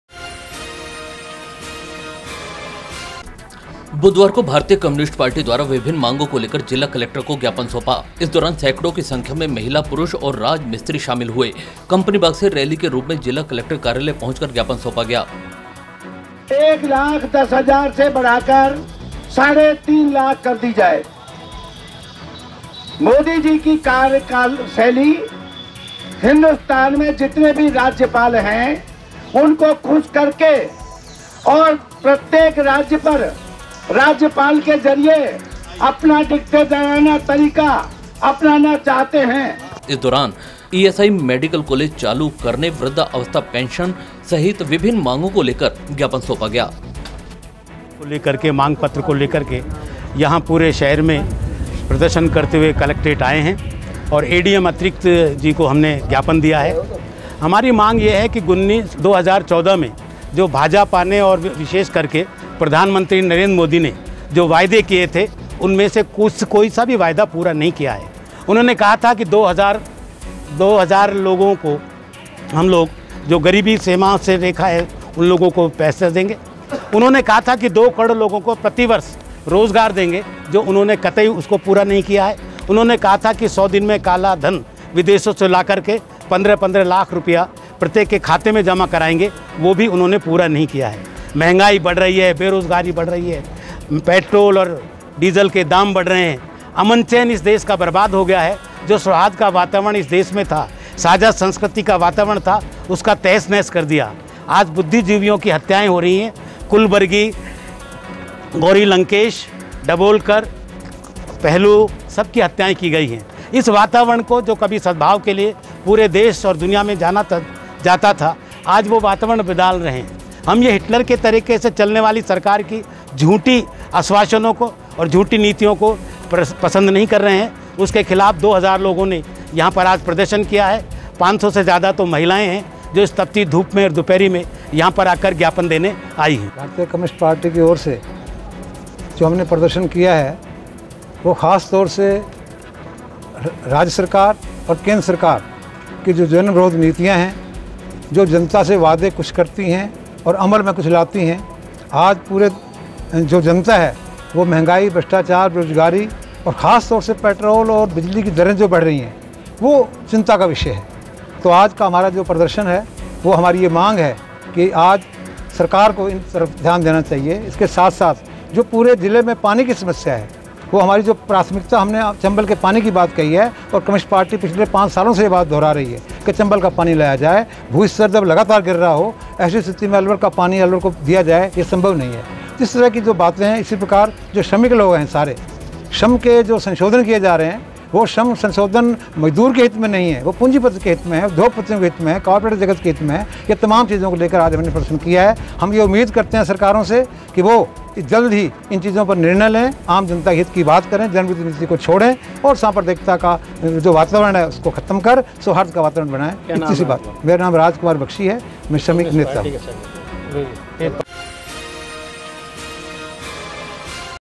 भारतीय कम्युनिस्ट पार्टी ने कई मांगो को लेकर दिया ज्ञापन, कंपनी बाग से जिला कलेक्टर कार्यालय तक निका
-भारतीय कम्युनिस्ट पार्टी ने कई मांगो को लेकर दिया ज्ञापन, कंपनी बाग से जिला कलेक्टर कार्यालय तक निकाली रैली